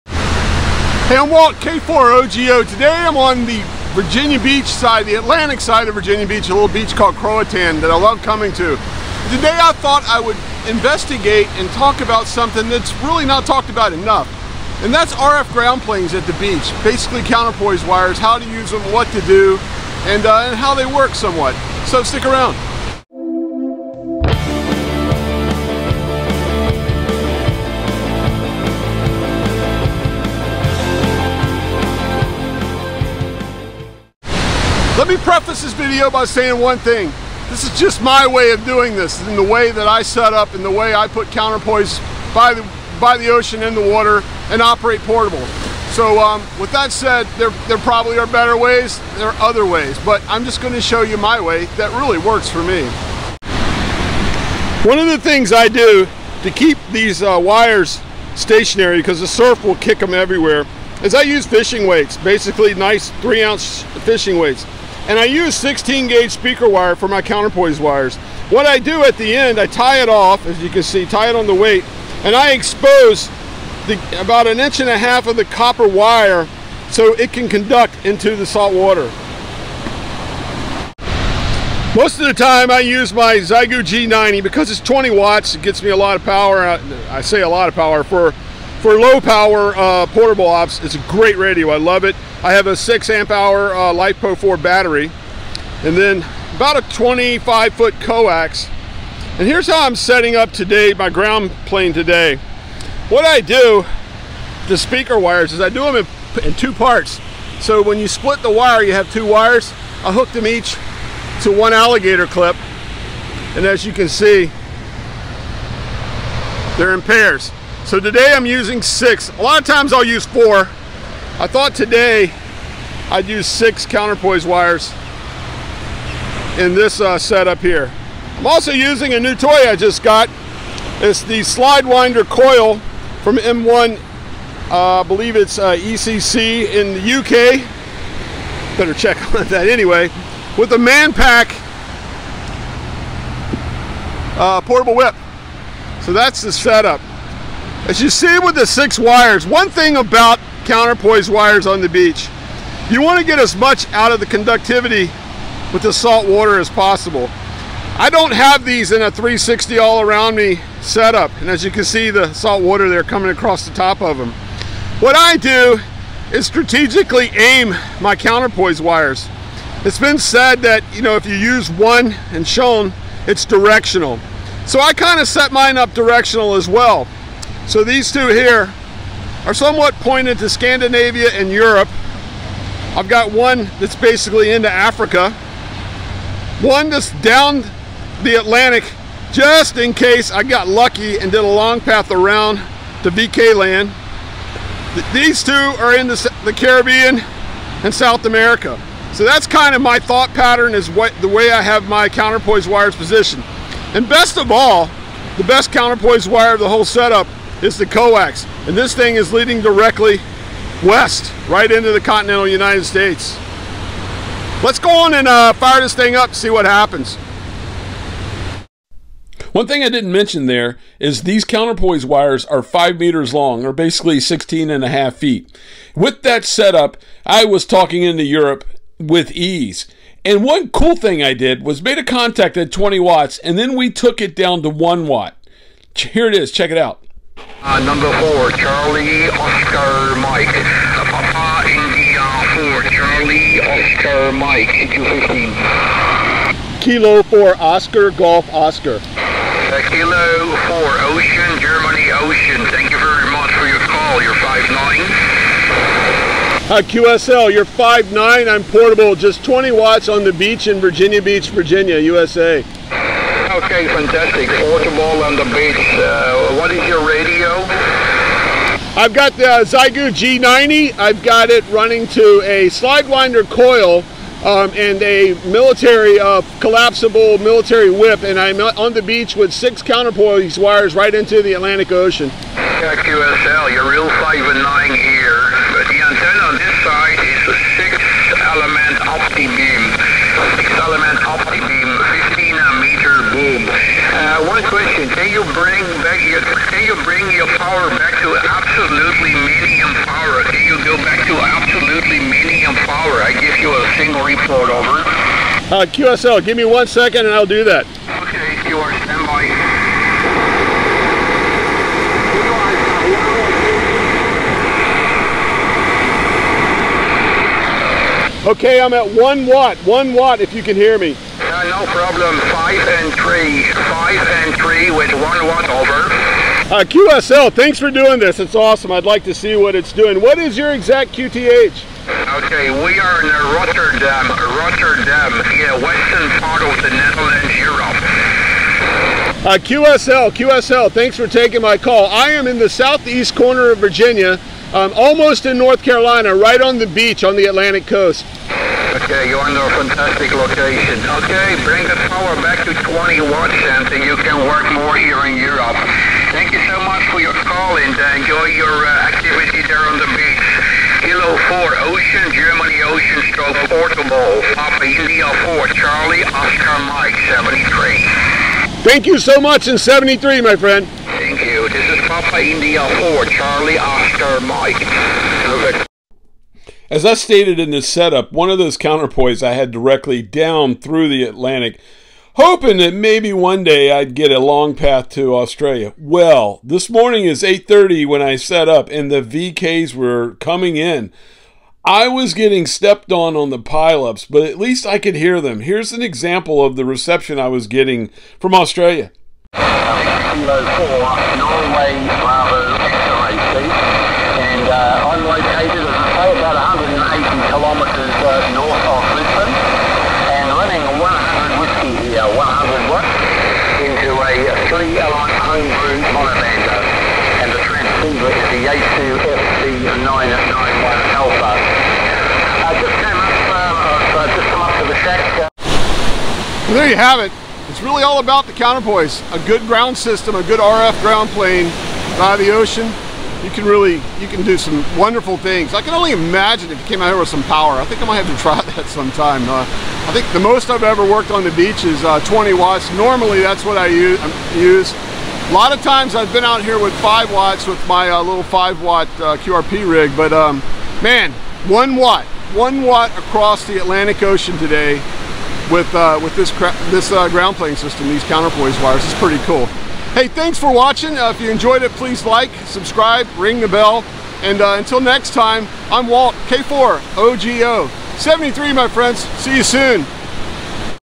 Counterpoise Wires at the Beach - How I Set Mine Up
This is how I setup my counterpoise wires when working portable on the beach.